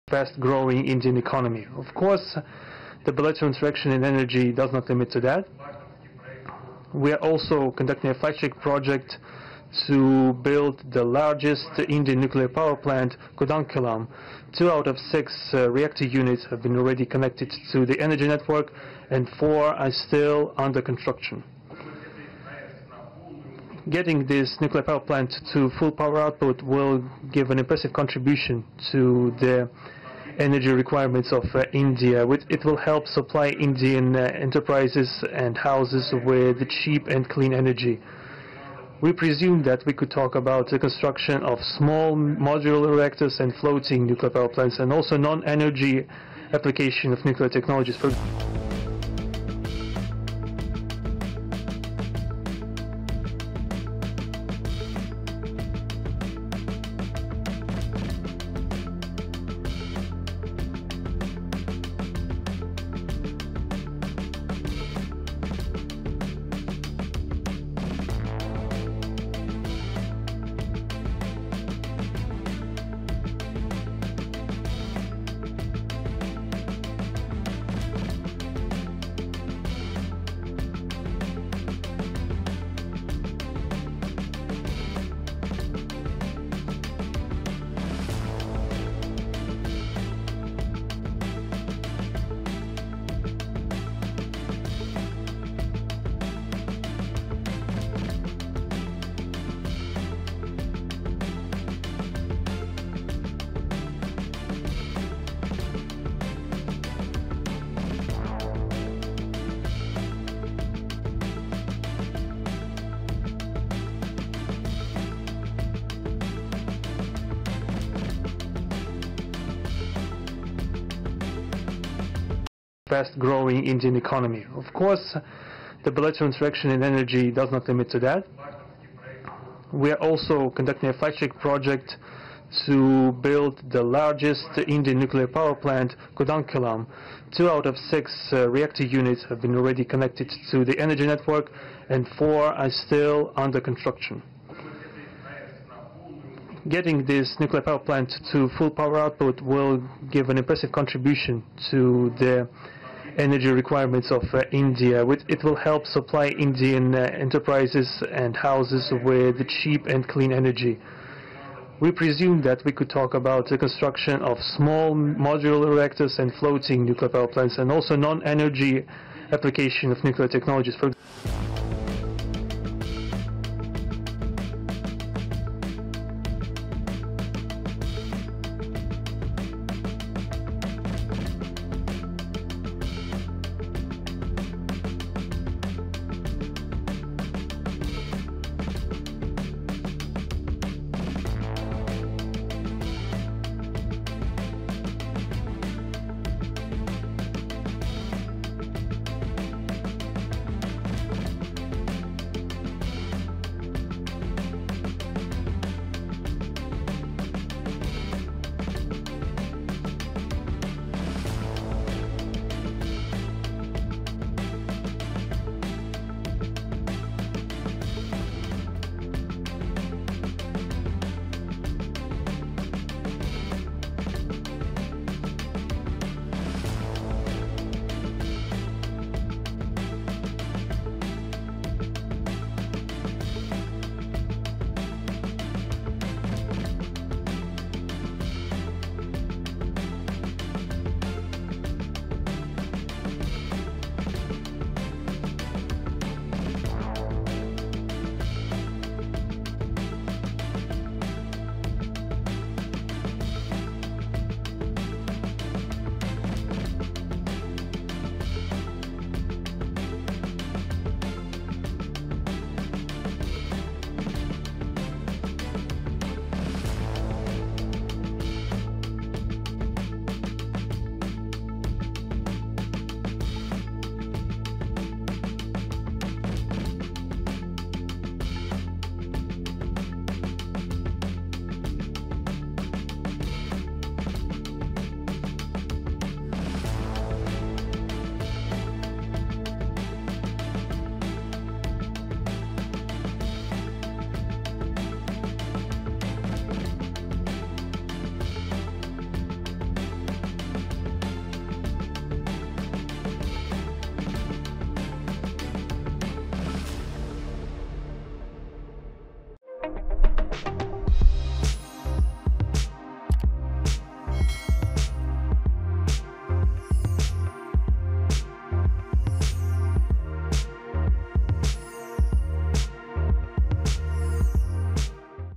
India-Russia Nuclear Pact Creates Panic In US? Putin, Modi Make Mega Atomic Announcement
Russian President Vladimir Putin announced major progress in India’s largest nuclear project at Kudankulam, confirming two active reactors and four more underway. He highlighted that full capacity will significantly boost India’s clean, affordable energy supply for industries and households. Putin also pitched small modular reactors and floating nuclear plants, areas where Russia holds global leadership. Rosatom is pushing to deepen cooperation, offering both SMR deployment and advanced large reactors. Recent high-level meetings in Mumbai reviewed ongoing projects, including Russia’s floating SMR successes and China’s demonstration reactor. India sees SMRs as vital for remote regions as it targets 100 GWe nuclear capacity by 2047. Watch